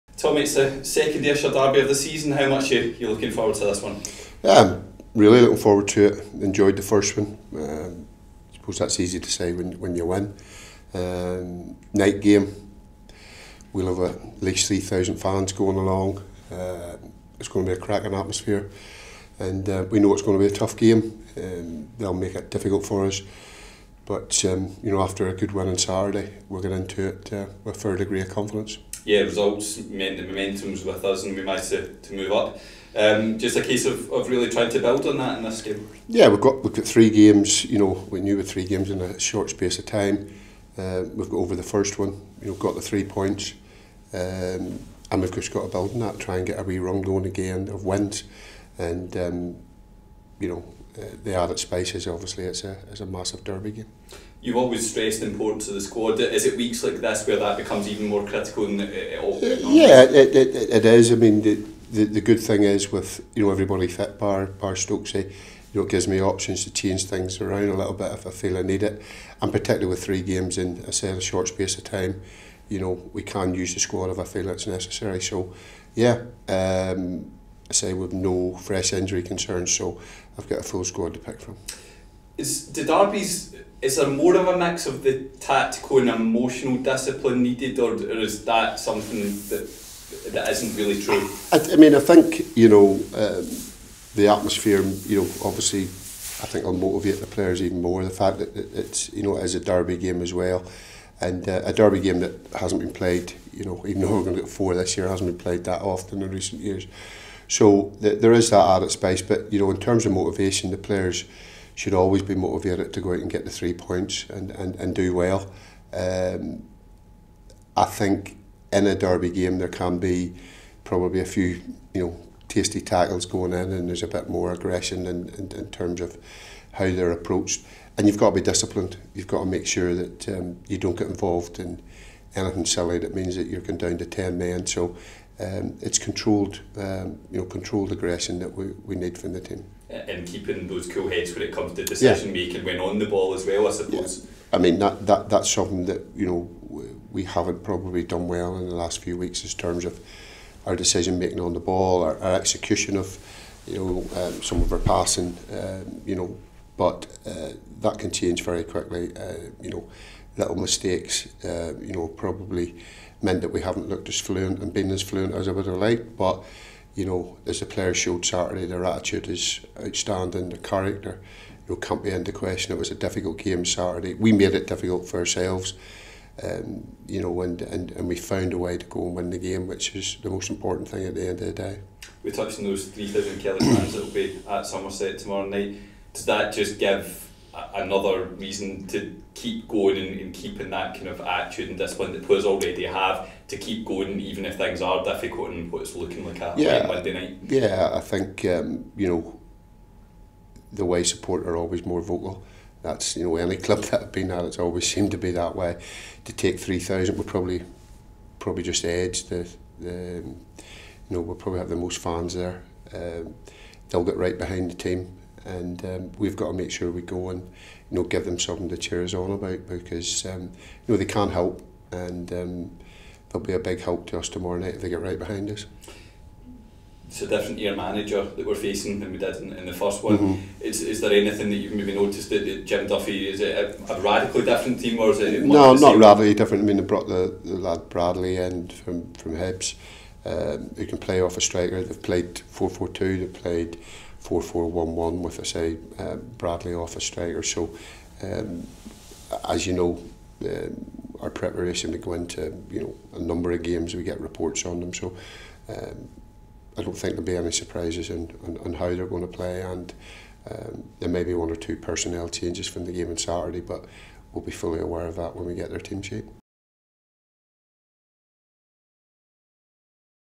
🎙 Ready for an Ayrshire derby cracker | Tommy Wright
The manager previewed our trip to Somerset